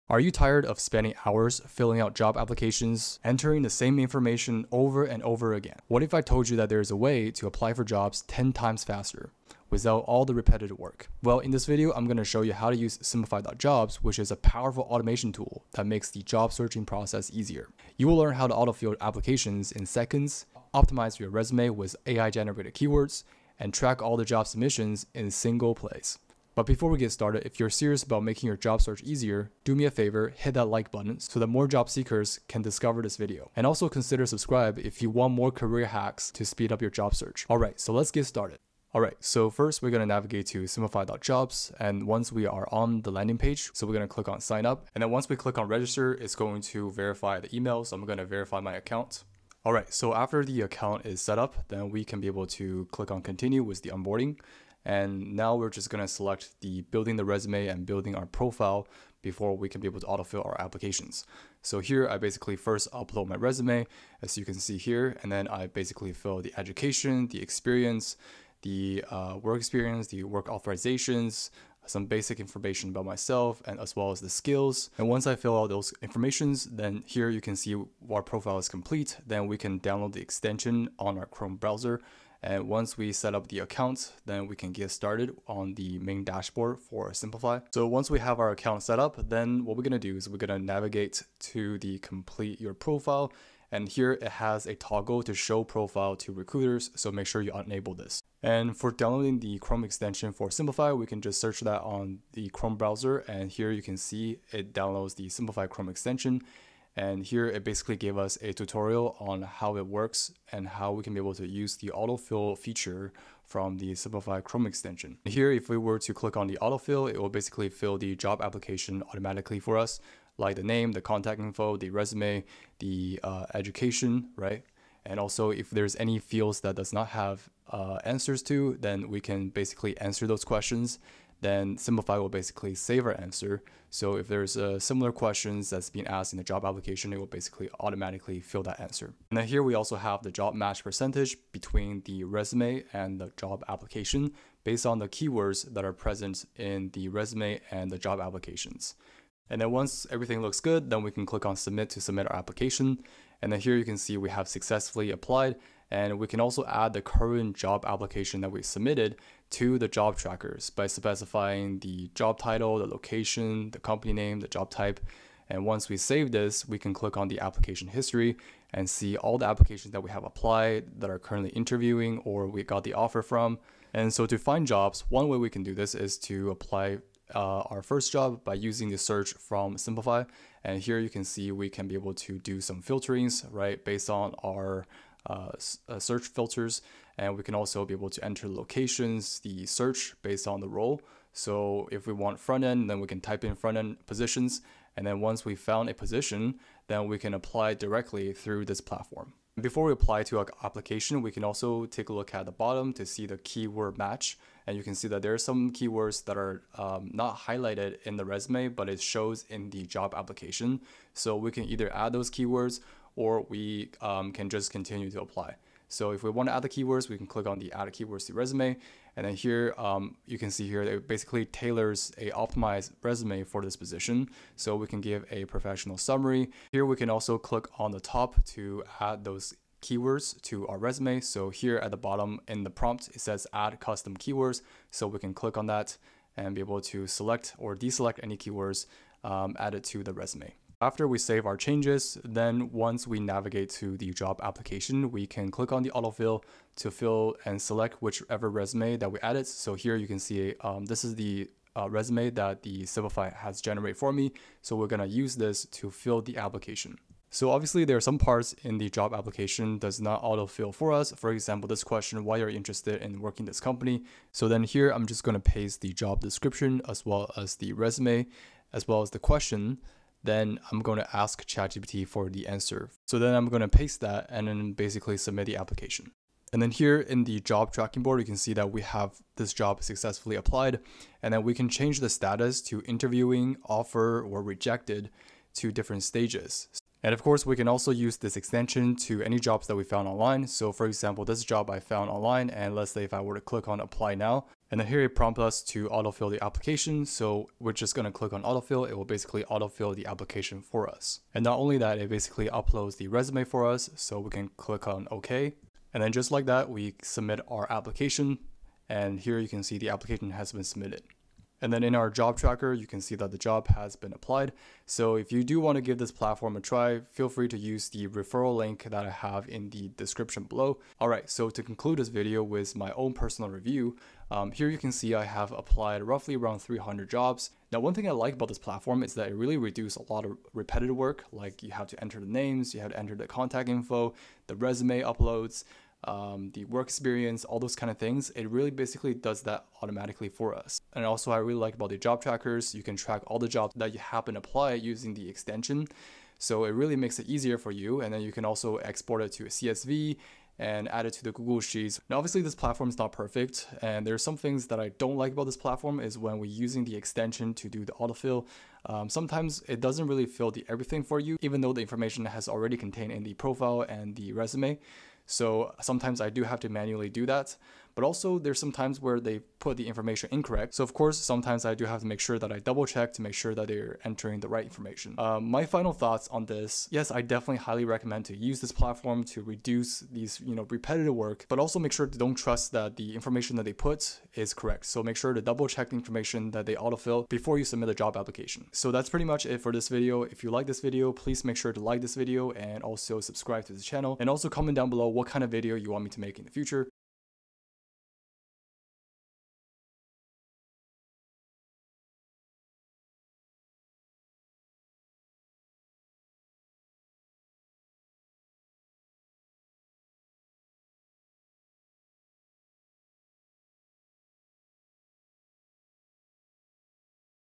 Apply to Jobs Faster with Simplify jobs (Job Search Hack 2025)
Tired of filling out job applications manually?
In this video, I show you how to use Simplify.jobs, a free tool that helps you apply to roles up to 10X faster by:

✔ Auto-filling applications across job portals
✔ Optimizing your resume and cover letter
✔ Tracking your job search in one place
✔ Suggesting roles based on your skills

Whether you're applying through Indeed, LinkedIn, or company websites, this tool speeds up the process and reduces repetitive work so you can focus on finding the right role—not clicking the same buttons over and over.

⚠️ I’ll also share the pros and cons, so you know exactly when Simplify.jobs is helpful and when manual application may still be better.
🔗 RESOURCES & LINKS

⏱️ Timestamps:

00:00 - Introduction: Apply to jobs 10X faster
00:38 - Simplify.jobs & how does it work?
05:21 - Final thoughts: Good & Bad of using Simplify.jobs